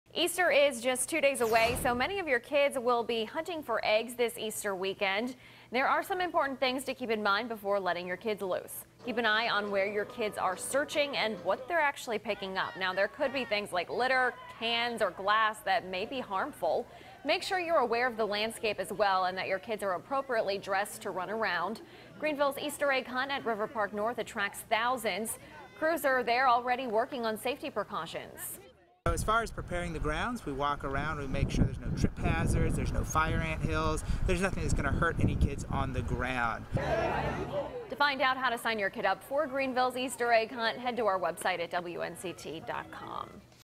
Keeping your kids safe at Easter egg hunts
Easter is just two days away, so many of your kids will be hunting for Easter eggs this weekend.